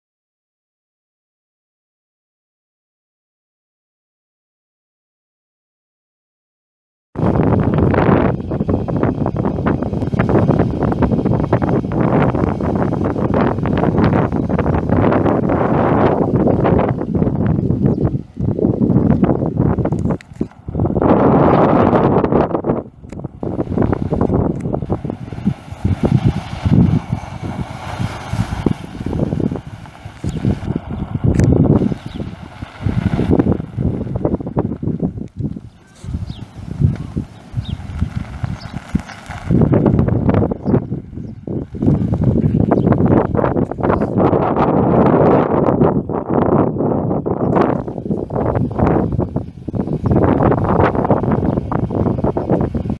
2006 Dodge Durango SUV for sale | no-reserve Internet auction July 5, 2017
2006 Dodge Durango SUV
198,834 miles on odometer
4.7L V8 SOHC 16V gas engine
Automatic transmission
Tilt
Cruise
AM/FM/CD
Power windows, locks, mirrors, and seat
Cloth interior
Fog lights
Running boards
Receiver hitch
245/70R17 tires
Oklahoma title
1D4HB48N86F113112
It's located in Hooker, OK, and we're selling it to the highest bidder regardless of price.